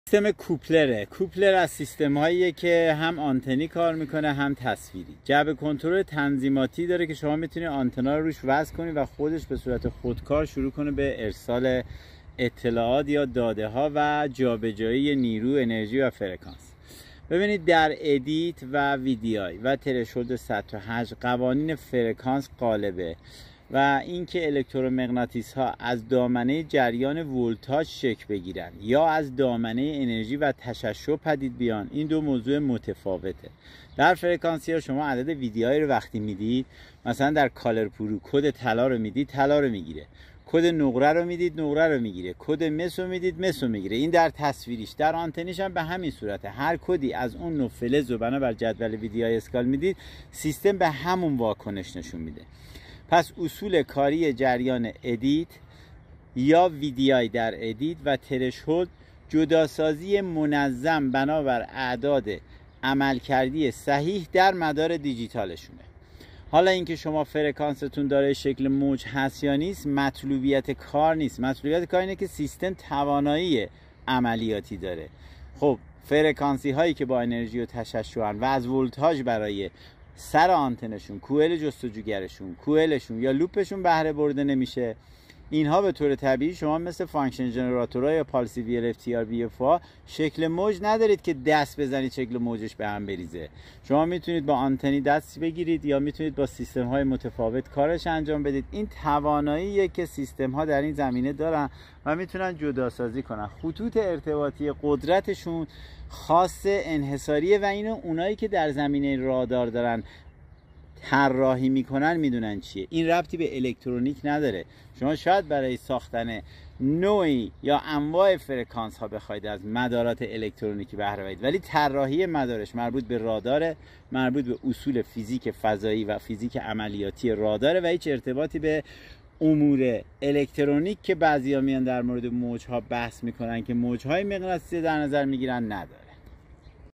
فلزیاب تفکیک دار تصویری فرکانسی کوپلر copler
عمق ، شعاع :
عمق : 35 متر
شعاع : 1500 متر

نوع عملکرد :
به صورت تصویری فرکانسی میتوان نقطه زنی تصویری یا اسکن تصویری انجام داد.
به صورت آنتنی راداری با جفت آنتن مجزا میتوان شعاع زنی و نقطه زنی آنتنی نمود.

نوع سیستم کاری :
فلزیاب کوپلر از سری فلزیاب های فرکانس القايی و جذبی می باشد.

نوع برنامه تصویری :
با برنامه سه لکه رنگی یا ویژولایزر سیمیلاتور (V3D) عمل مینماید. و با تعیین تنظیمات قبل از تصویر گیری و فیلتر و آنالیز بعد از تصویر گیری میتوان نوع منابع و ساختار ها را بنابر تعیین تنظیمات در ادیت (EDIT) مشخص نمود.

با برنامه تفکیک رنگی عددی یا کالرپرو (COL PRO) عمل مینماید. و با تعیین تنظیمات قبل از تصویر گیری و فیلتر و آنالیز بعد از تصویر گیری میتوان اهداف و منابع مختلف را با کد عدد وی دی ای (VDI) در برنامه جداسازی و تفکیک و آنالیز نمود.

قدرت تفکیک :
تفکیک بنابر جدول وی دی ای اسکال را دارا میباشد.
اهداف و فلزات از جمله طلا ، نقره ، حفره ، برنز ، نیکل ، مس ، روی ، آهن ، برنج ، آلومینیوم ،کروم و … را بصورت مجزا جداسازی و تفکیک مینماید.
در این نوع سیستم ها در جهت تفکیک اهداف و جداسازی منابع مزاحم ذرات آلیاژ فلزات و مواد معدنی می باشد.

تنظیمات مداری :
» ادیت (EDIT) : بنابر جدول وی دی ای اسکال میباشد و با تعیین کد مربوط به هر هدف یا فلز مورد نظر میتوان آن هدف یا فلز را بصورت مجزا از سایر منابع تشخیص داده و جداسازی نمود.

» ترشهولد (THRESHOLD) :  تنظیمات ابعاد هدف (IND SIZE) یا سطح و حجم ترشهولد در سیستم های راداری جهت مشخص نمودن ابعاد هدف مورد نظر اپراتور یا جداسازی منابع هم شکل و همگن کاربرد دارد.

» تراک اسپید (TRACK SPEED) : تنظیم سرعت علائم مزاحم از علائم مطلوب و همچنین سرعت رفع موانع را مشخص مینماید.

» کورش (COARSE) : در جهت رفع و حذف موانع زمین و پارازیت های غیر مفید ، در زمین های سفت و سخت مورد استفاده قرار میگیرد.
» فاین (FINE) : در جهت افزایش توان سیستم در زمین های یکدست کاربرد دارد.

» تراک (TRACK) : رفع موانع زمین در زمانی که تغییرات ناگهانی در زیر زمین پدید می آید را کنترل می نماید.

» وی دی ای دیسک (VDI DISC) : تنظیم میزان تفکیک و تشخیص بنابر اعداد VDI در جدول وی دی ای اسکال.

» وی دی ای سنس (VDI SENS) : تنظیم حساسیت و توانایی سیستم جهت حذف منابع مزاحم.

سنسور های فرکانسی :
دارای مادون قرمز یا لیزر میباشد که داشتن مادون قرمز یا لیزر توان سیستم فلزیاب را برای تشخیص شرایط طلا یا فلزات تعیین شده در عدد وی دی ای (VDI) افزایش می دهد.

دارای سنسور التراسونیک یا امواج مافوق صوت در جهت ایجاد کانال مدوله برای تاثیر هر چه کمتر موانع مزاحم و افزایش قدرت در ارسال و بازتاب علائم میباشد.

دارای کوئل کربن فرکانس دوقطبی خاص با توانایی ارسال دو طبقه همزمان فرکانس در جهت افزایش قدرت و کیفیت سیستم میباشد.

سایر توضیحات :
در فلزیاب تصویری کوپلر COPLER  تصویر گیری مستقیم با خود سیستم و کوئل الکترونیکی لیزری انجام میگیرد.
سیستم آنتنی بصورت بیسیم و مجزا قابلیت اضاف نمودن و نصب برروی سیستم را دارا ست.

با تک آنتن میتوان شعاع زنی و نقطه زنی نموده و با جفت آنتن اضاف میتوان یکسری عملیات های مربوط به جفت آنتن را به انجام رسانید.

دسته آنتن بی سیم اضافه برای عملیات کار با جفت دسته آنتن برای شعاع زنی AC و نقطه زنی DC می باشد.
نیاز به متصل نمودن خوراک یا نمونه فلز یا هدف نداشته بلکه با تعیین عدد VDI به راحتی میتواند طلا GOLD یا آن فلز یا هدف تعیین شده را تشخیص داده و مشخص نماید.

برای عملیات جستجو در مناطقی که دارای مواد معدنی و منابع سنگی و درهم میباشد کارایی دارد.

تنظیمات فلزیاب تصویری کوپلر COPLER در جهت تشخیص منابع و جداسازی اهداف اعم از سیلیس ، ذرات ، فلزات هم طبقه ، فلزیاب همگن ، فلزیاب آلیاژی ، منابع مزاحم و فلزات مختلف بصورت مجزا میباشد.

قدرت زیاد عمق در این سیستم ها نشات گرفته از فرکانس میباشد.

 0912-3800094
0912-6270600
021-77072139